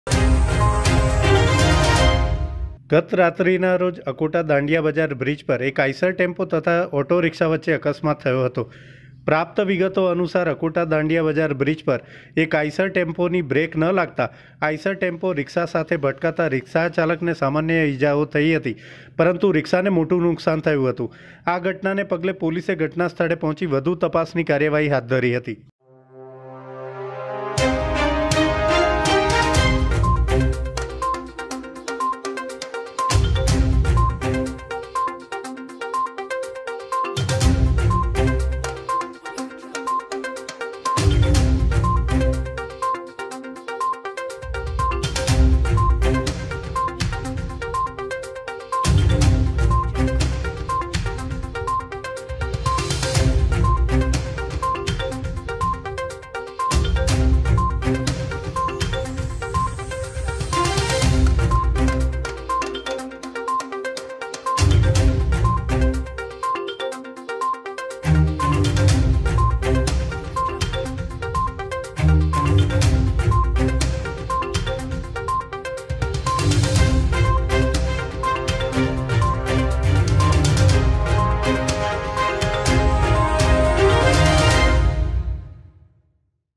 Zulm ki Awaz Newsગતરોજ શહેરના અકોટા દાંડિયાબજાર બ્રીજ પર એક આઇસર ગાડી અને રીક્ષા વચ્ચે અકસ્માત થયો હ
ગતરોજ શહેરના અકોટા દાંડિયાબજાર બ્રીજ પર એક આઇસર ગાડી અને રીક્ષા વચ્ચે અકસ્માત થયો હતો.

પ્રાપ્ત માહિતી અનુસાર અકોટા દાંડિયાબજાર બ્રિજ પર ગતરાત્રીના સમયે એક આઇસર ગાડીની બ્રેક ન લાગતા આઇસર ગાડી ઓટો રીક્ષા સાથે ભટકતા રીક્ષાને મોટું નુકશાન થયું હતું જ્યારે સદનસીબે રીક્ષાચાલકનો સામાન્ય ઇજાઓ બાદ બચાવ થયો હતો આ ઘટનાને પગલે પોલીસે ઘટનાસ્થળે પહોંચી આગળની કાર્યવાહી હાથ ધરી હતી